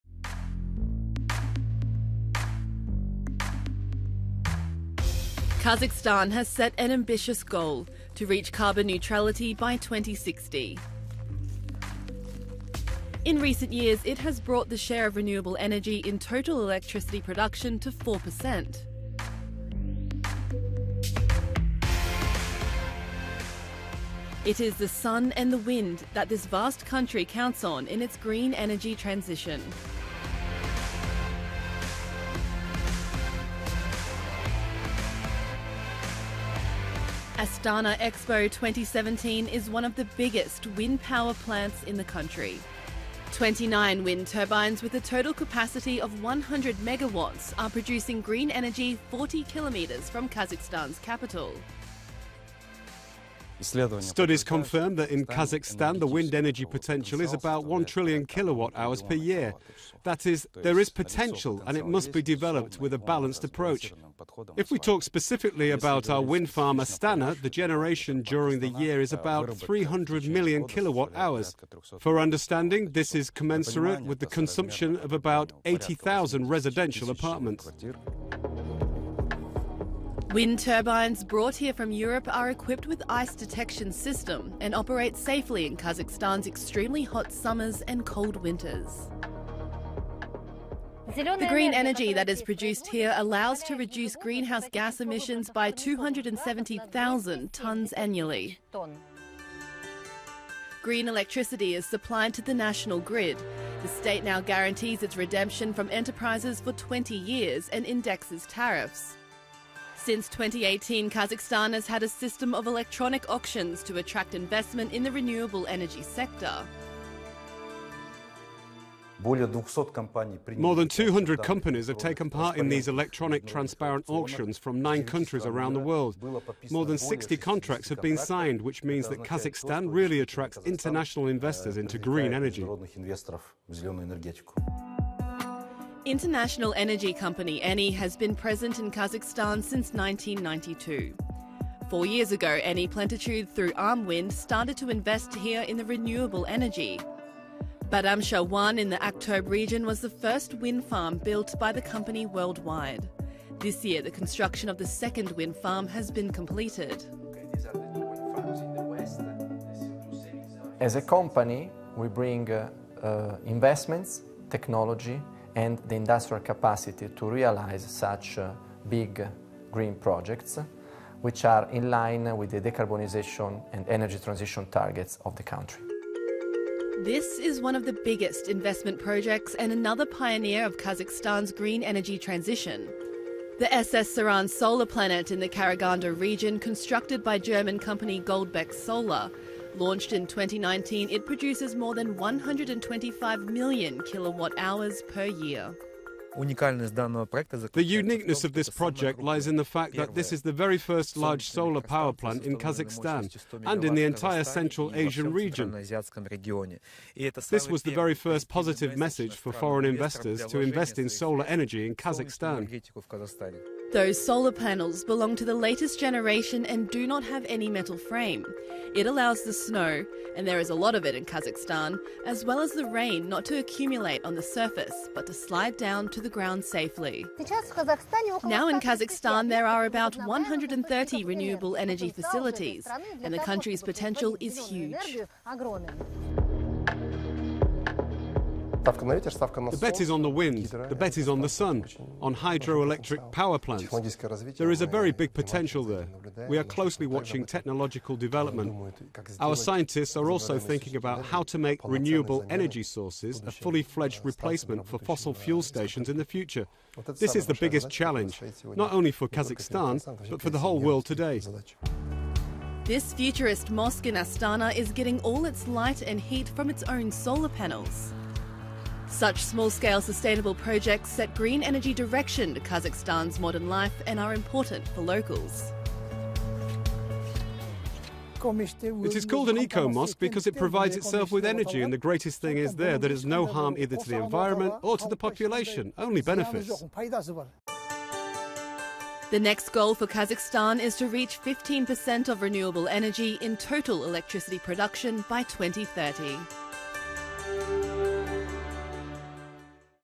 Betting on renewables: can Kazakhstan become carbon neutral by 2060?
In recent years Kazakhstan has brought the share of renewable energy in total electricity production to 4 percent. Kazakhstan has now established around 130 renewable energy facilities, and the country's potential for green energy generation is huge.